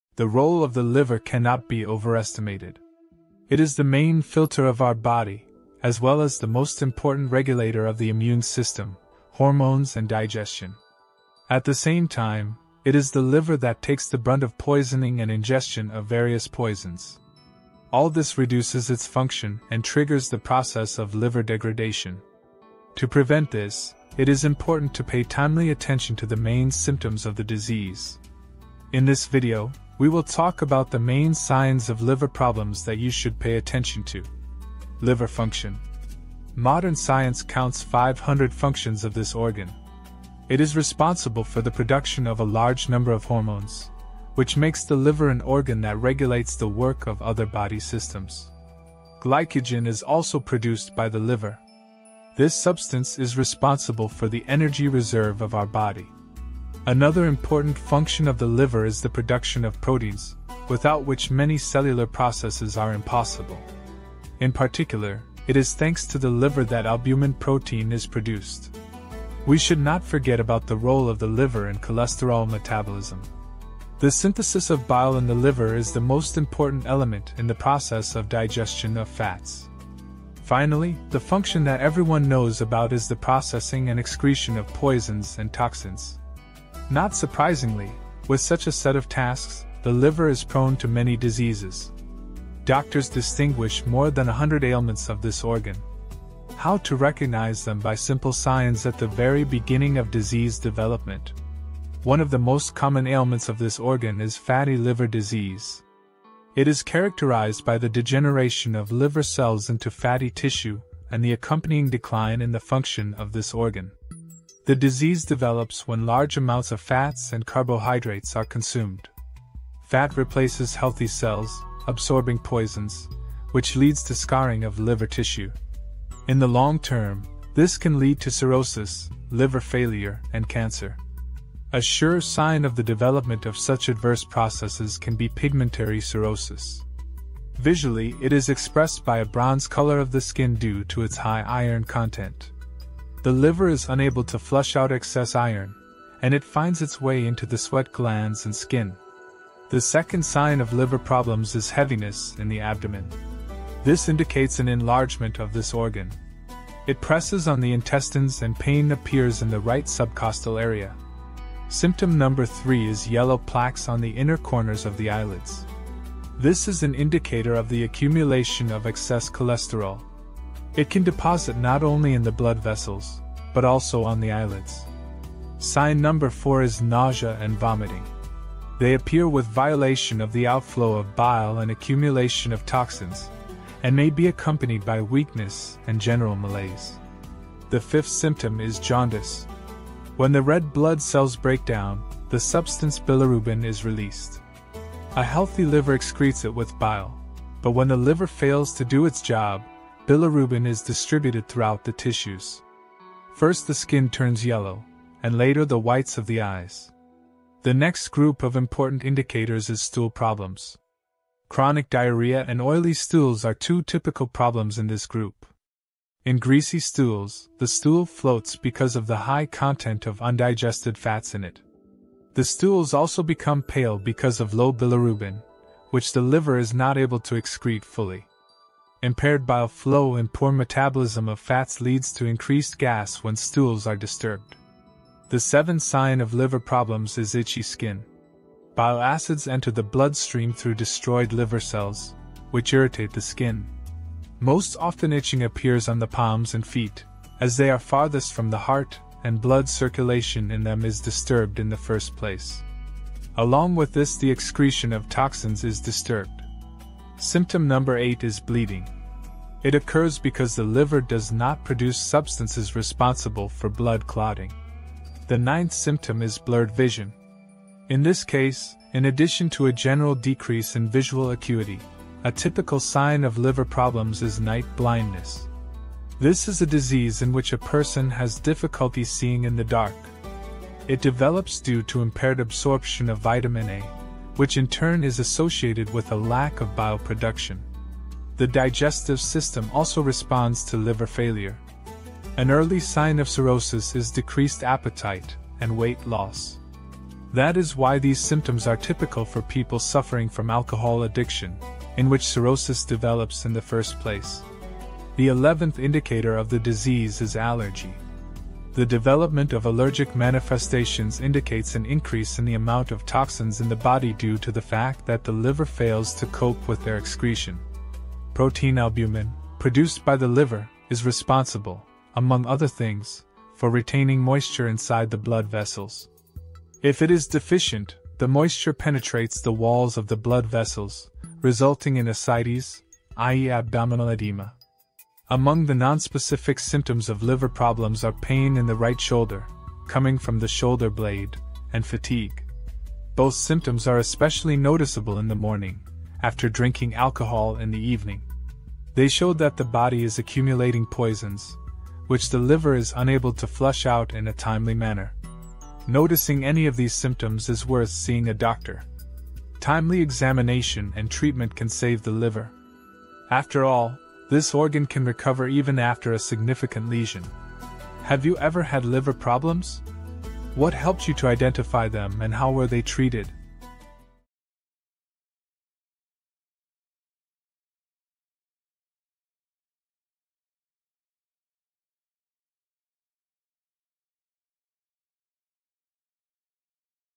Recognized Husband's Disease in Time and Saved Him from the Hospital! Symptoms of Liver Disease
The role of the liver cannot be overestimated.
It is the main filter of our body, as well as the most important regulator of the immune system, hormones and digestion.
At the same time, it is the liver that takes the brunt of poisoning and ingestion of various poisons.
All this reduces its function and triggers the process of liver degradation.
To prevent this, it is important to pay timely attention to the main symptoms of the disease.
In this video, we will talk about the main signs of liver problems that you should pay attention to.

➤➤➤DEWARNING!
This channel is not medical! All recommendations are taken from public sources and must be strictly agreed with your doctor!!!

Health to you and Longevity!